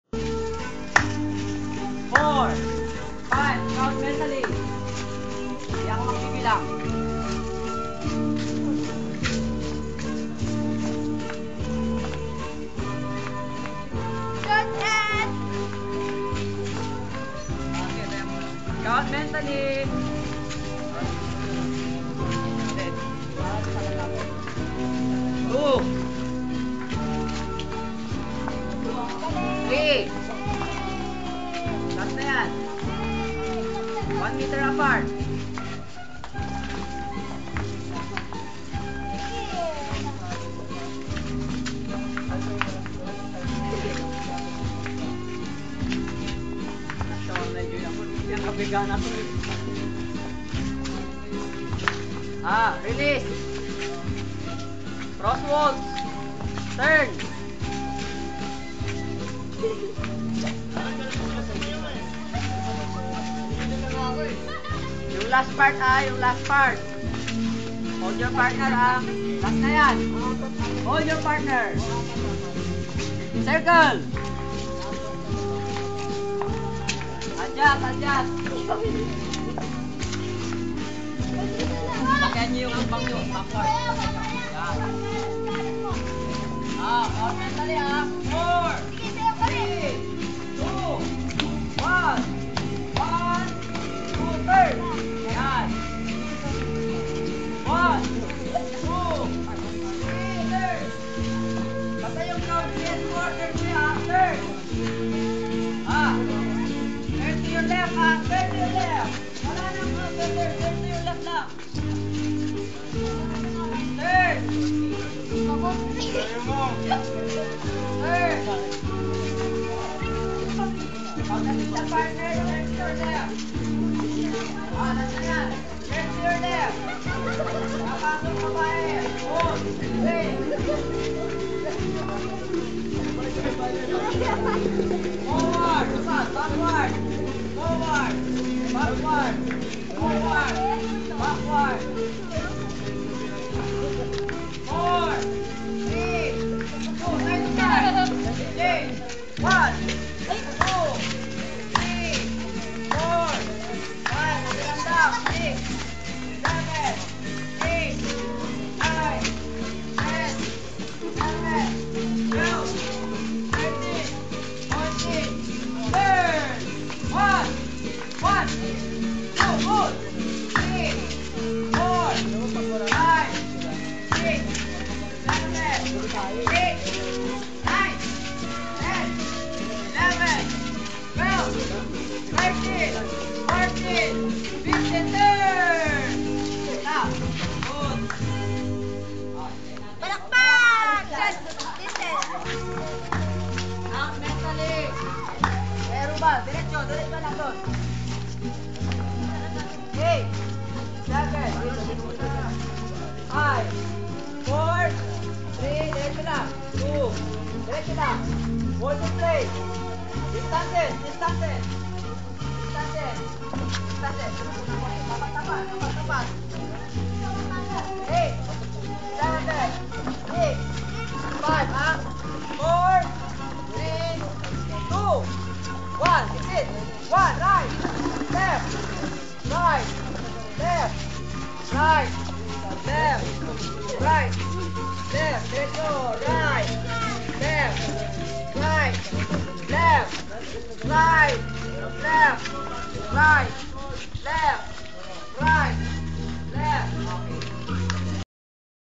Waltz Practice
This is a video of a Waltz Practice for my cousins debut. I'm wearing a dark blue shirt (hard rock cafe shirt) and white jogging pants and pink shoes (skechers). My partner (which is my cousin and the debutant's brother) is wearing a yellow tank top and blue w/orange shorts.